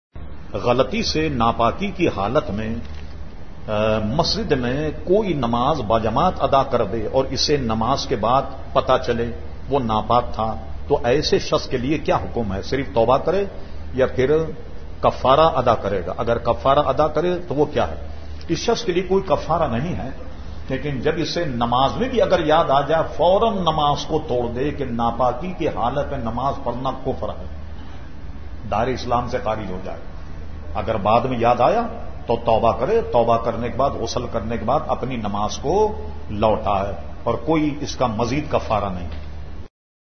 GHALATI SE NAPAKI KI HALAT ME NAMAZ PADHLI TO KYA KAREIN ? ANSWERED BY SYED SHAH TURAB UL HAQ QADRI
Listen Question & Answer Session Series of Huzoor Syed Shah Turab ul Haq Qadri رحمة الله عليه
Aala Hazrat ka Sipahi - Barelvi Social Media Network